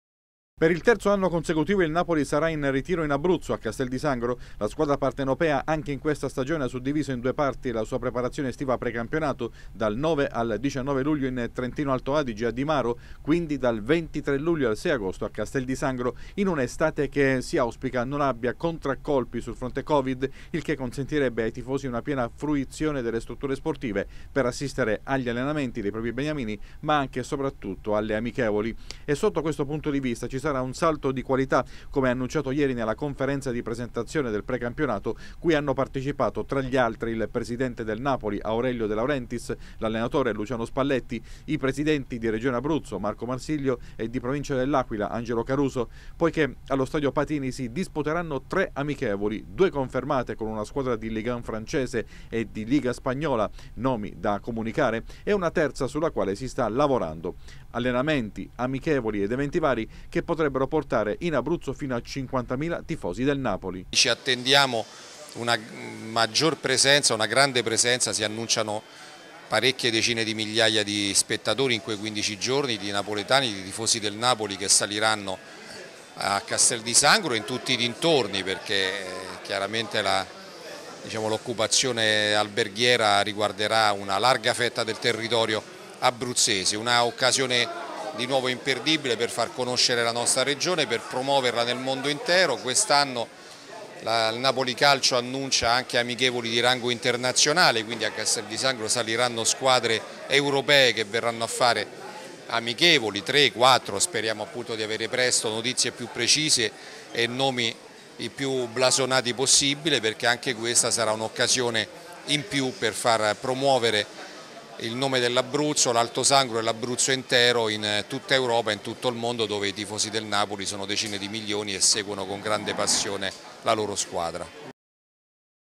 Ritiro Napoli a Castel di Sangro: attesi migliaia di tifosi
Dal 23 luglio al 6 agosto prossimi il Napoli tornerà per il terzo anno consecutivo in ritiro a Castel di Sangro. Previste anche tre amichevoli di livello internazionale che potrebbero portare in Abruzzo migliaia di tifosi